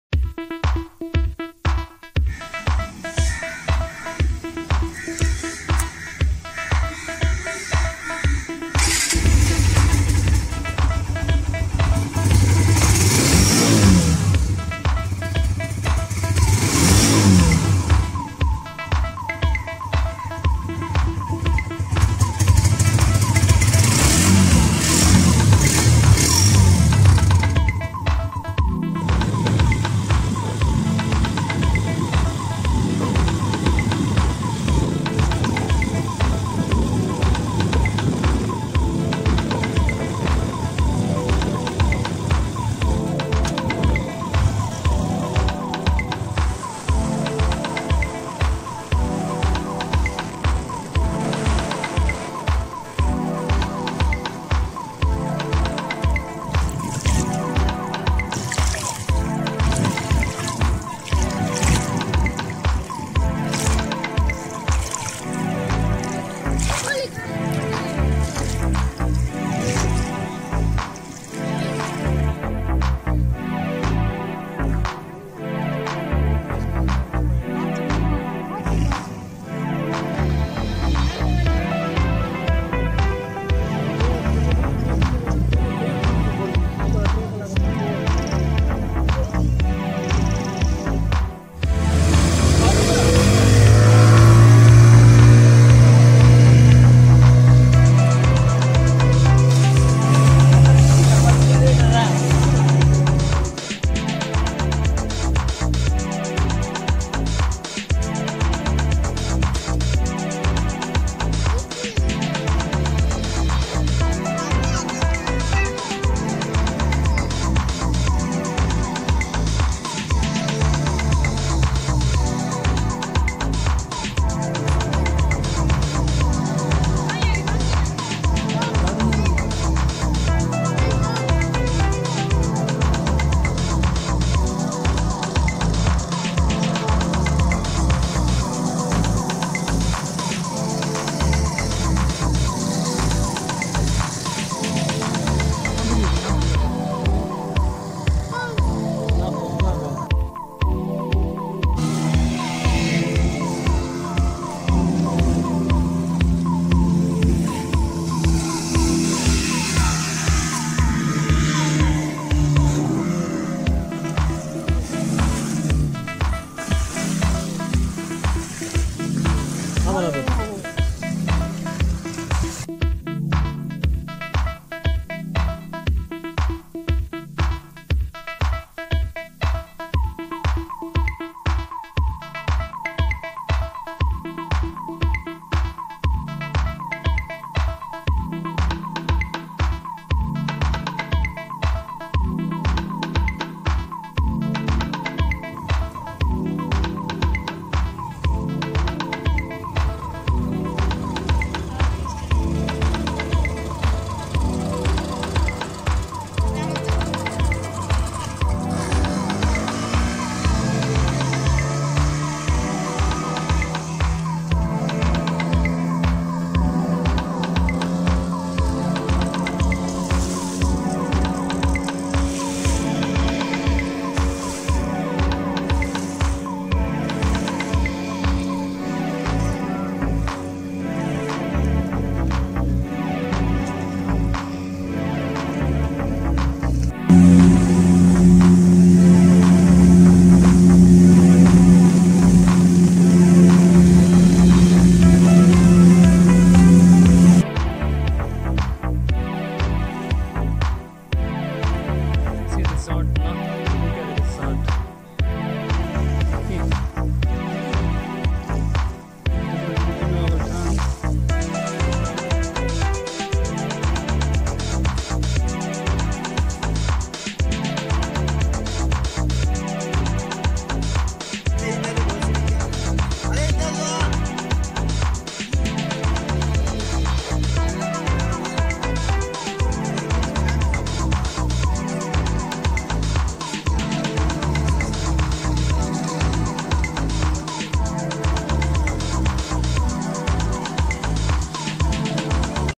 chotiari Dam tour | chotiyari dam fish | sanghar food point | bike tour on Honda CB150f
سنڌ جي ضلعي سانگهڙ۾ ھڪ سياحتي ھنڌ چوٽياريون ڊيم گھمڻ جي راء پنھنجي موٽرسائيڪل تي ڪراچيءَ کان سانگھڙ